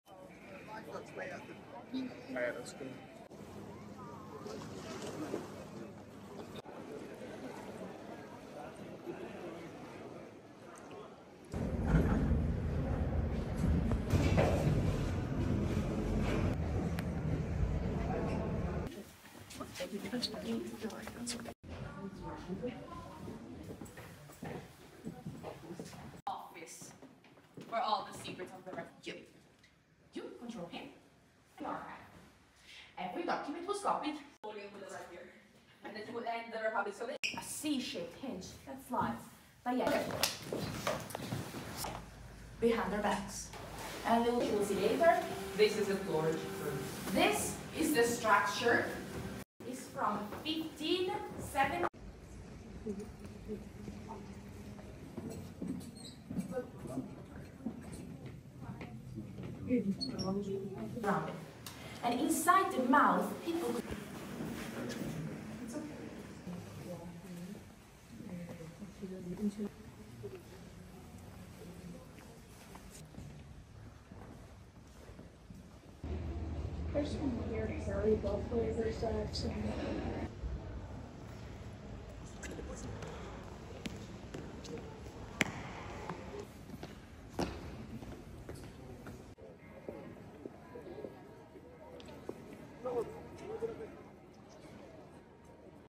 Doge's Palace, Palazzo Ducale
Doge's Palace in Venice, Palazzo Ducale historic government lavish headquarters of the Venice Republic.